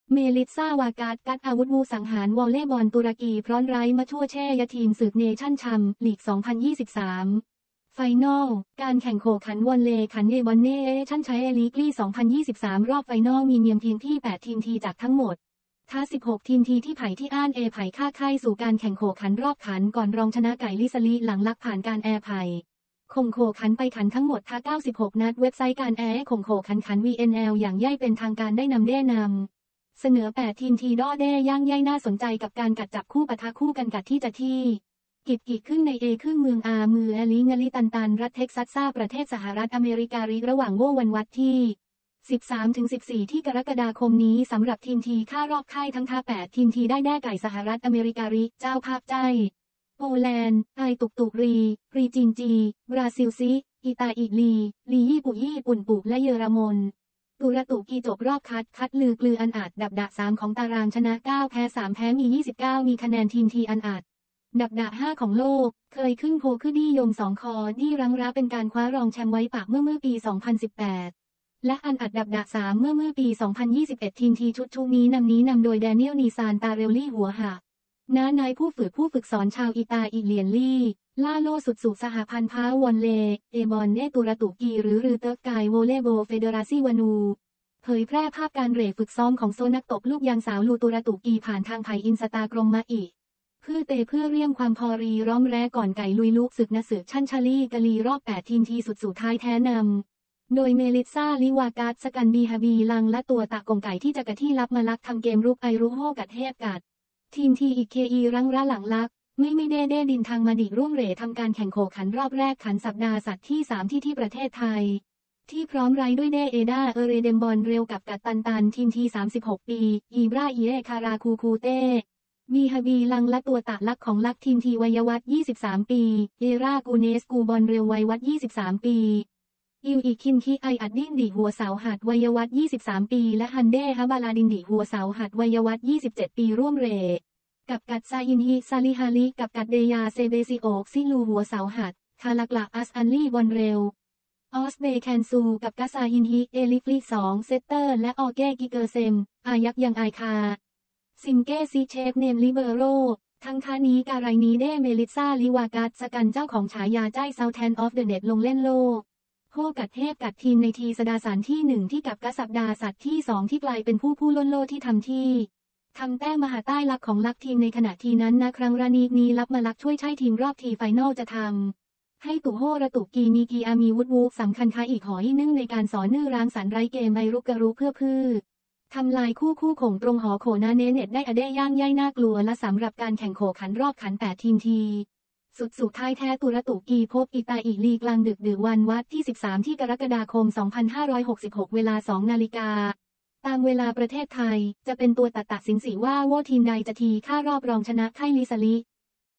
เมลิสซา วาร์กัร์ กัส อาวุธวุ สังหารวอลเลย์บอลตุรกี พร้อร้ มช่วช่ ยทีมศึกเนชันชั ส์ ลีก2023 ไฟนอลส
เมลิสซา วาร์กัร์ กัส อาวุธวุ สังหารวอลเลย์บอลตุรกี พร้อร้ มช่วช่ ยทีมศึกเนชันชั ส์ ลีก
2023 ไฟนอลส